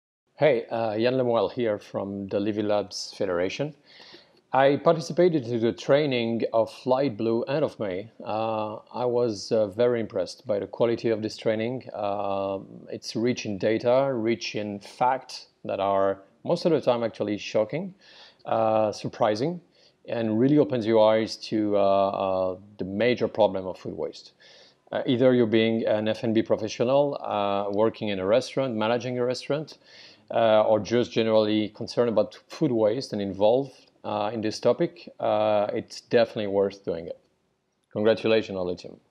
Yann LeMoel, Managing Partner at Pufferfish Partners
Want to take a deep dive into the burning issue of FOOD WASTE?
Join our online self paced Master Class :  Food Waste Prevention and the $700 Billion Opportunity, for professionals keen to take action.

Successful participants receive a certificate of proficiency to boost up their career!

Yann LeMoel, Managing Partner at Pufferfish Partners has successfully completed our Full Course on Food Waste Prevention. Let's hear what he would like to say about the Full Course.